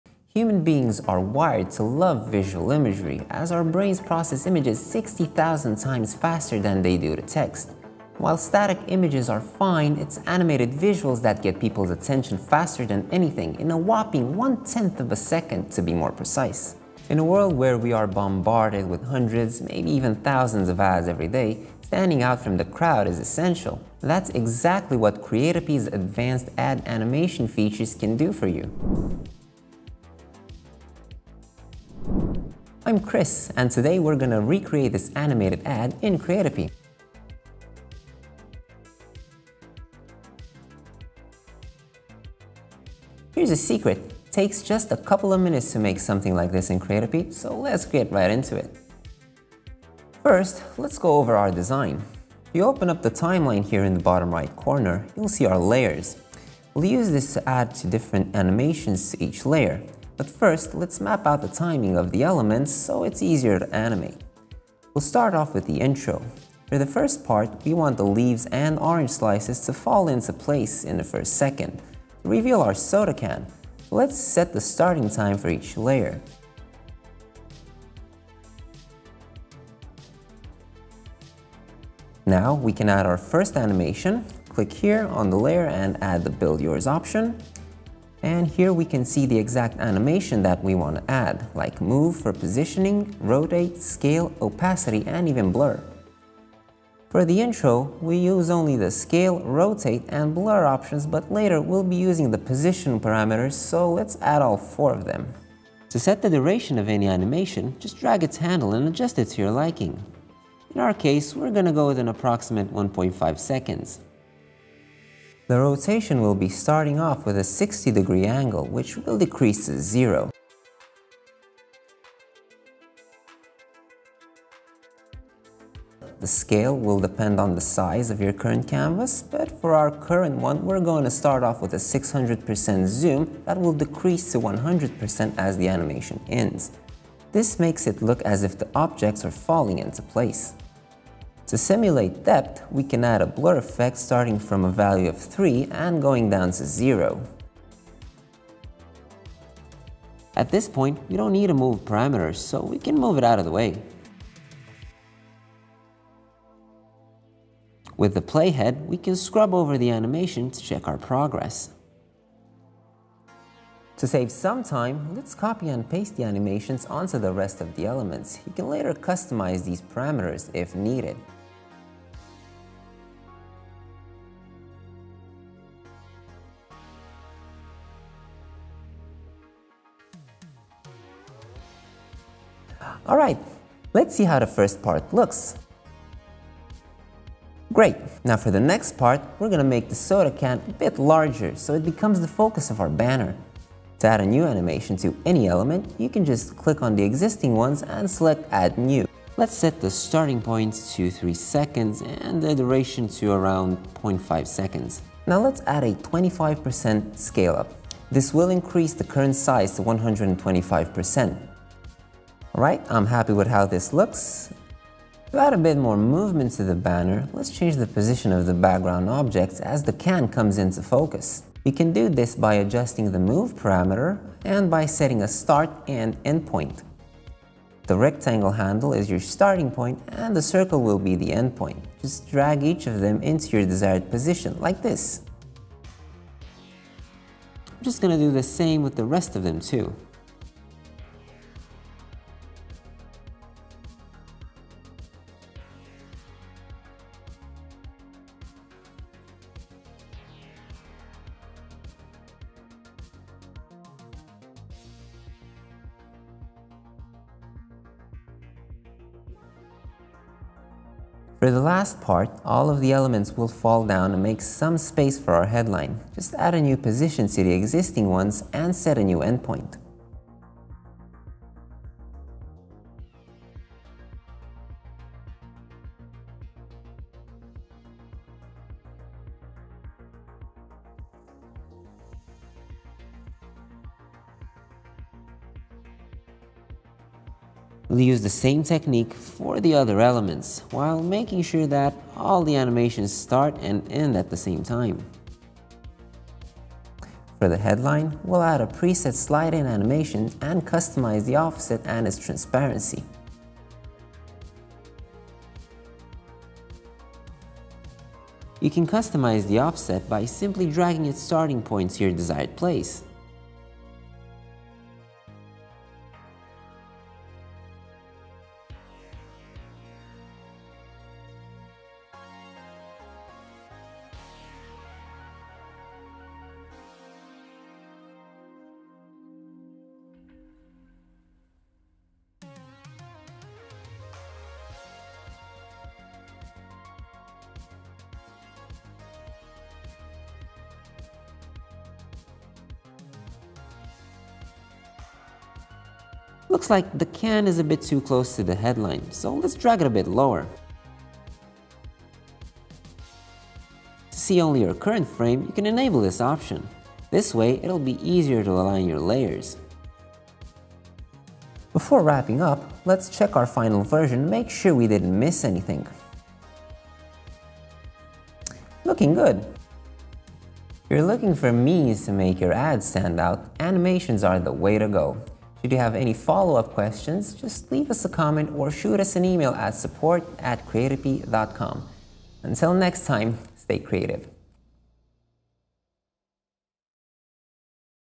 How To ANIMATE Your Ads in Creatopy
We've been noticing a lot of customers asking for more control over animations in Creatopy. Well, we listened and came with a new solution for you. It will give you a greater freedom of creating beautiful stories with professional animations, without any coding skills needed whatsoever./ /The new animations will give you total control over the individuality of your ads - and will place much more power in your hands. This video explains how the new features work. Watch this video to find out how you can use the new feature in your ads.